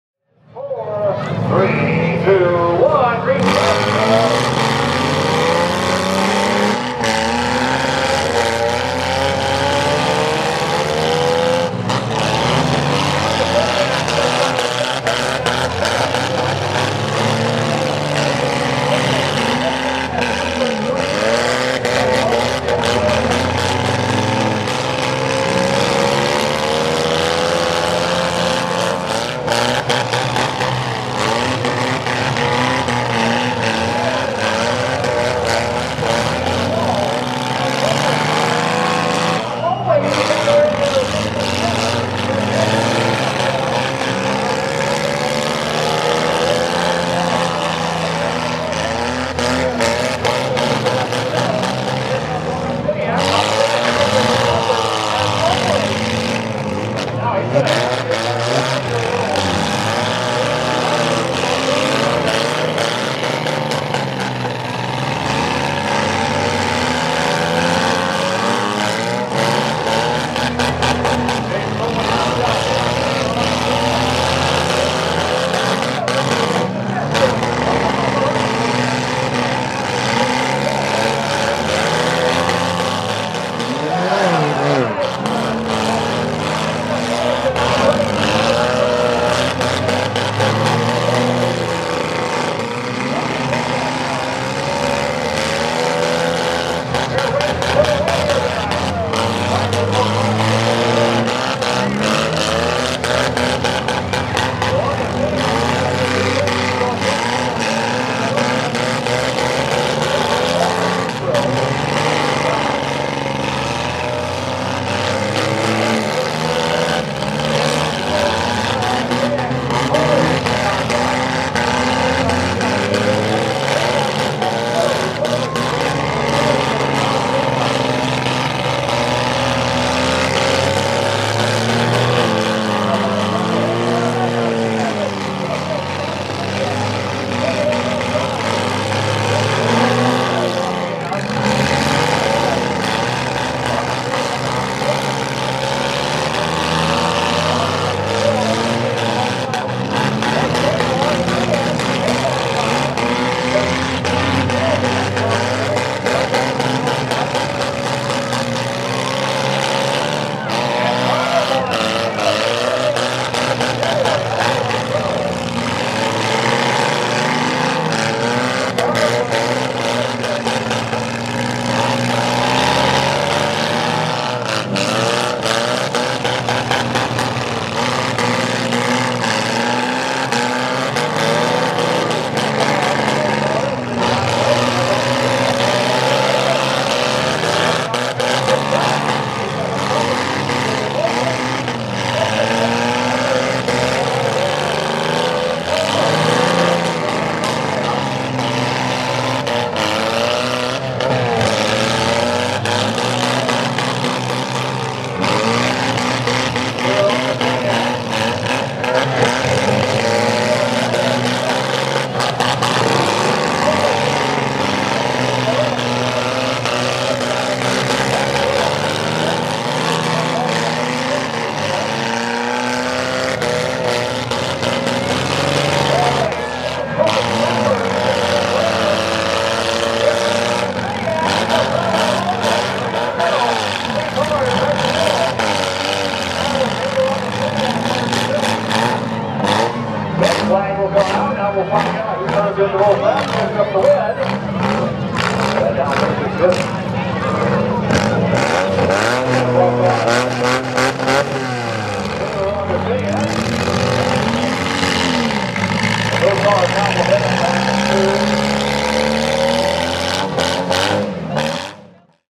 Figure 8 Feature - Monroeville Derby 2022
Video footage of the 2022 Monroeville Demolition Derby. Took place in Monroeville Indiana on August 5th & 6th. Promoted by "Monroeville".